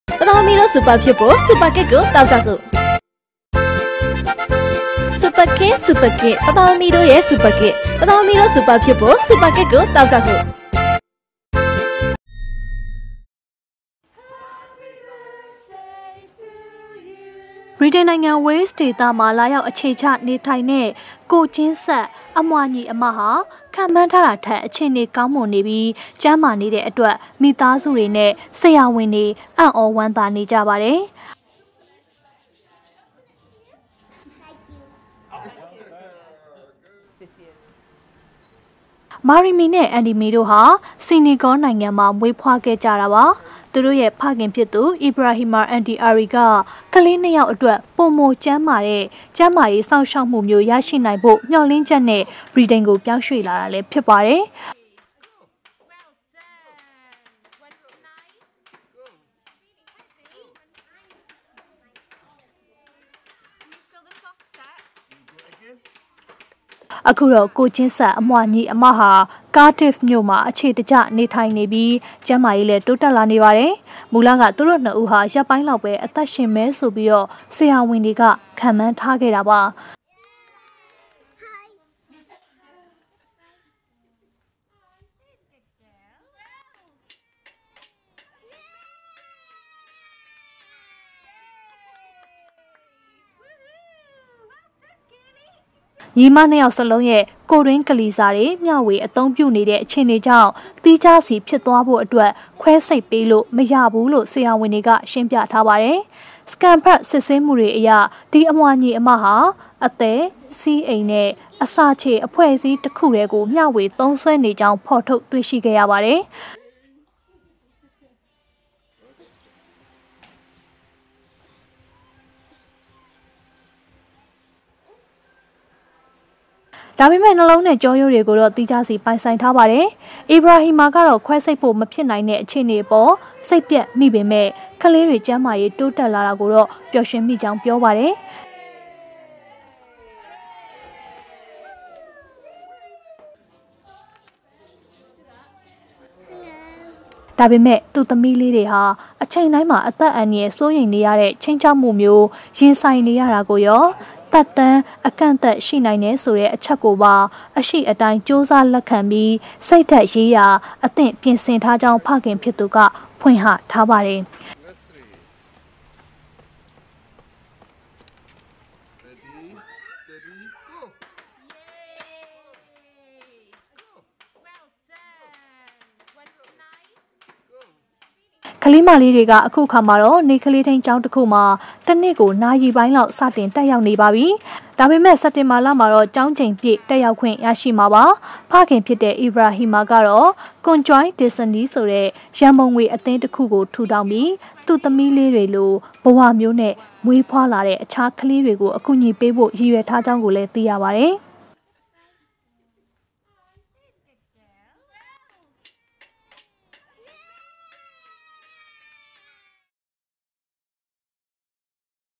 ေမွ်ာ္လင့္ထားတာထက္ က်န္းမာ ေပ်ာ္ရႊင္ေနတဲ့ ကိုယ္ခ်င္းဆက္ အႁမႊာ ညီအစ္မ
သြဂုတ် - ၇၊ ၂၀၁၉

ဗြိတိန်နိုင်ငံ၊ ဝေးလ်စ်ဒေသမှာ လာရောက် အခြေချ နေထိုင်တဲ့ ကိုယ်ချင်းဆက် အမြွှာ ညီအစ်မဟာ ခန့်မှန်းထားတာထက် အခြေအနေ ကောင်းမွန်ပြီး ကျန်းမာနေတဲ့အတွက် မိသားစုတွေနဲ့ ဆရာဝန်တွေ အံ့ဩ ဝမ်းသာနေကြပါတယ်။

မာရီမီနဲ့ အန်ဒီမီတို့ဟာ ဆီနီဂေါ နိုင်ငံမှာ မွေးဖွားခဲ့ကြတာပါ။ သူတို့ရဲ့ ဖခင်ဖြစ်သူ အီဘရာဟီမာ အန်ဒီအာရီက ကလေး နှစ်ယောက်အတွက် ပိုမို ကောင်းမွန်တဲ့ ကျန်းမာရေး စောင့်ရှောက်မှုမျိုး ရရှိနိုင်ဖို့ မျှော်လင့်ချက်နဲ့ ဗြိတိန်ကို ပြောင်းရွှေ့လာတာ ဖြစ်ပါတယ်။
အခုတော့ ကိုယ်ချင်းဆက် အမြွှာညီအစ်မဟာ ကားဒစ်ဖ်မြို့မှာ အခြေတကျ နေထိုင်နေပြီး ကျန်းမာရေးလည်း တိုးတက်လာနေပါတယ်။ မူလက သူတို့ နှစ်ဦးဟာ ရက်ပိုင်းလောက်ပဲ အသက်ရှင်မယ်လို့ ဆရာဝန်တွေက ခန့်မှန်းထားခဲ့တာပါ။

ညီအစ်မ နှစ်ယောက်စလုံးရဲ့ ကိုယ်တွင်း ကလီစာတွေ မျှဝေ အသုံးပြုနေကြတဲ့ အခြေအနေကြောင့် သီးခြားစီ ဖြစ်သွားအောင် ခွဲစိတ်ပေးလို့ မရကြောင်း ဆရာဝန်တွေက ရှင်းပြပါတယ်။ Scanဖတ် စစ်ဆေးမှုတွေအရ ဒီအမြွှာ ညီအစ်မဟာ အသည်း၊ ဆီးအိမ်နဲ့ အစာချေ အဖွဲ့အစည်း တစ်ခုတည်းကို မျှဝေ သုံးစွဲနေကြောင်း ဖော်ထုတ် တွေ့ရှိခဲ့ပါတယ်။ ဒါပေမဲ့ နှလုံးနဲ့ ကျောရိုးတွေကိုတော့ သီးခြားစီ ပိုင်ဆိုင်ထားပါတယ်။

အီဘရာဟီမာကတော့ ခွဲစိတ်ဖို့ မဖြစ်နိုင်တဲ့ အခြေအနေအပေါ် စိတ်ပျက်မိပေမယ့် ကလေးတွေ ကျန်းမာရေး တိုးတက်လာတာကိုတော့ ပျော်ရွှင်မိတယ်လို့ ပြောပါတယ်။ ဒါပေမဲ့ သူ့သမီးလေးတွေဟာ အချိန်တိုင်း အသက်အန္တရာယ် စိုးရိမ်ရတဲ့ ခြိမ်းခြောက်မှုမျိုး ရင်ဆိုင်နေရတာကိုရော၊ သက်တမ်း အကန့်အသတ် ရှိနိုင်တယ်ဆိုတဲ့ အချက်ကိုပါ အရှိအတိုင်း ကြိုးစား လက်ခံပြီး စိတ်ဓာတ်ရေးရာ အသင့်ပြင်ဆင်ထားကြောင်း ဖခင်ဖြစ်သူက ဖွင့်ဟပါတယ်။

ကလေးမလေးတွေဟာ အခုအခါ နေ့ကလေးထိန်း ကျောင်းတစ်ခုမှာ တစ်နေ့ကို နာရီပိုင်းလောက် စတင် တက်ရောက်နေပါပြီ။ ဒါပေမဲ့ စက်တင်ဘာလမှာတော့ ကျောင်းချိန်ပြည့် တက်ရောက်ခွင့် ရကြမှာပါ။ ဖခင်ဖြစ်တဲ့ အီဘရာဟီမာကတော့ Conjoined Destiny ဆိုတဲ့ ရန်ပုံငွေ အသင်းတစ်ခုကို ထူထောင်ပြီး သူ့သမီးလေးတွေလို ဘဝမျိုးနဲ့ မွေးဖွားလာတဲ့ တခြားကလေးတွေကို အကူအညီပေးဖို့ ရည်ရွယ်ထားကြောင်း သိရပါတယ်။